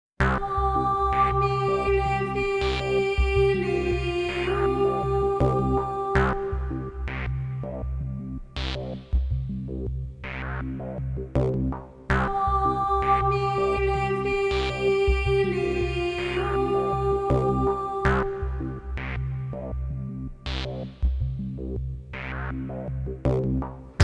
The multiverse acrylic on canvas original art
The idea that every thought creates new realities

‘If a dice is cast, there are six possible outcomes in the several distinct universes’
Hugh Everett